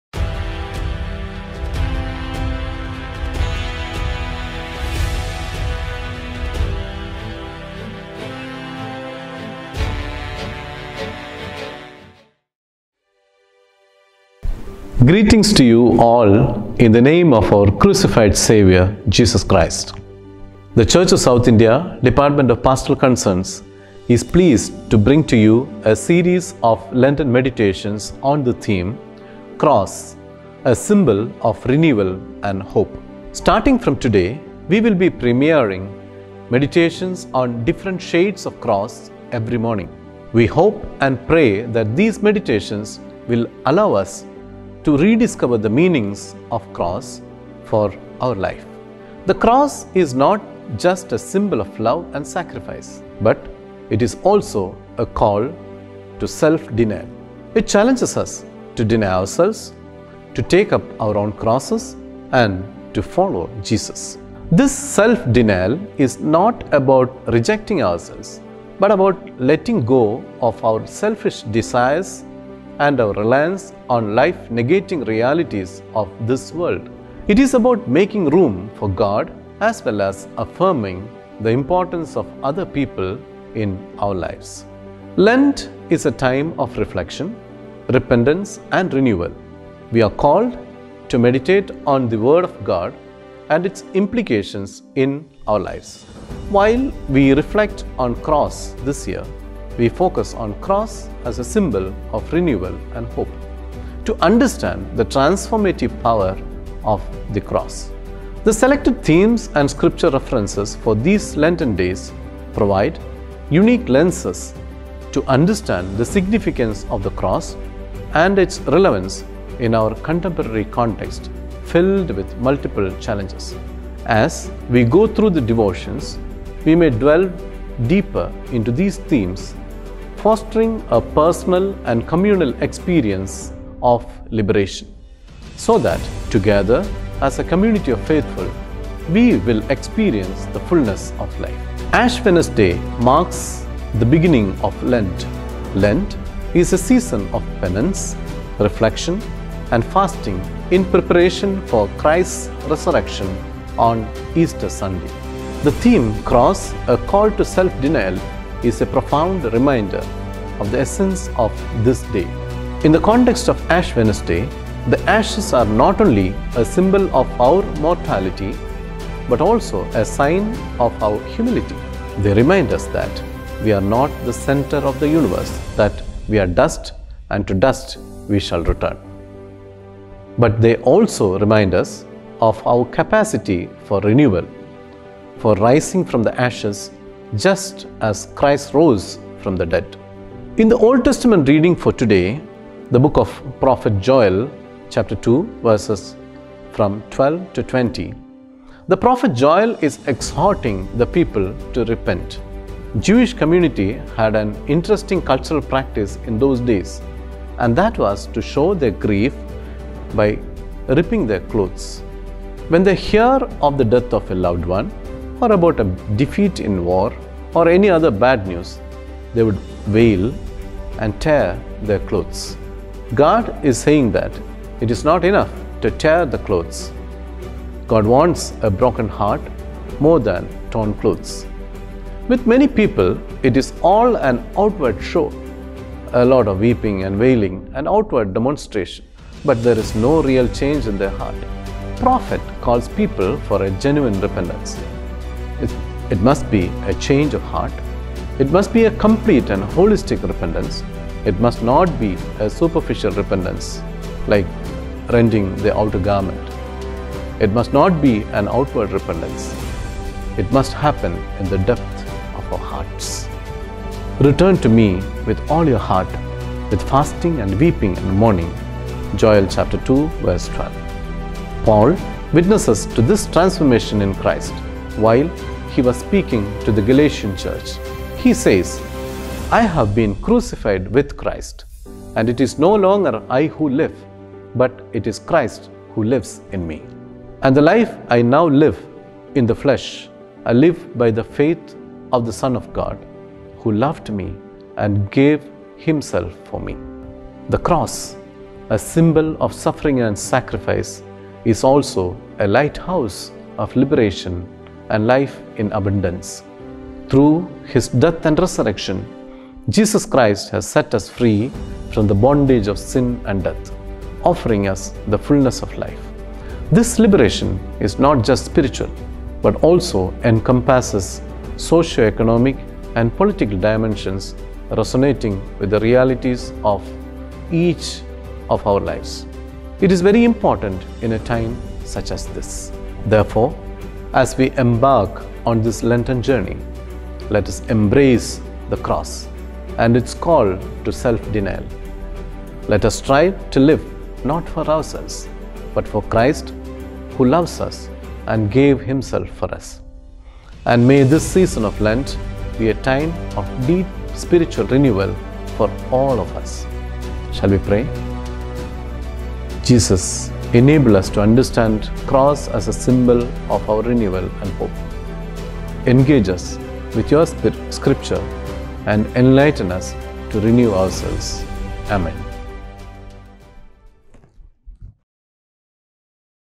Lenten Meditations 2024 Day 1 (“ Cross: A Call to Self Denial” )
The Pastoral Aid Department of the Church of south India brings out devotions for the 40 Lenten Days. Watch Rev. Anilal M. Jose, Director Department of Pastoral Concerns, presents the Lenten meditation Day 1.